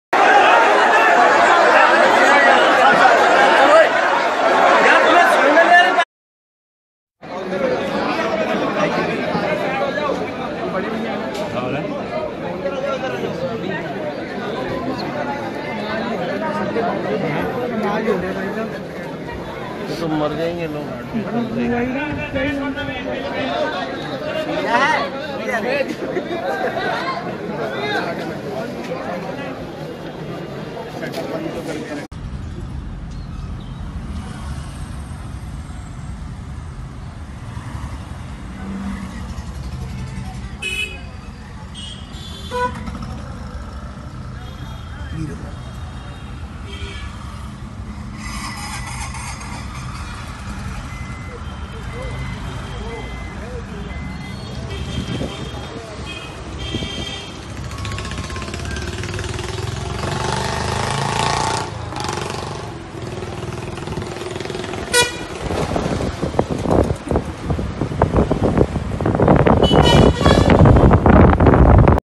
Amar colony sealing से परेशान वयापारी
Aar colony Sealing-Amar colony market sealing was taken by authorities on 8th March 2018, its an extensively taken on a large scale, where will all the people go associated with the shops, trade, as its a Large & famous market of Delhi.